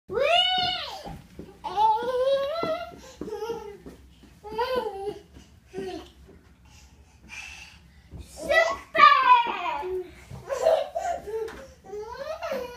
Arriving at Art Of Animation after driving from Toronto to Orlando
Arriving a Art Of Animation, Disney World. We had just driven from Toronto to Orlando so our kids had a lot of energy to burn off.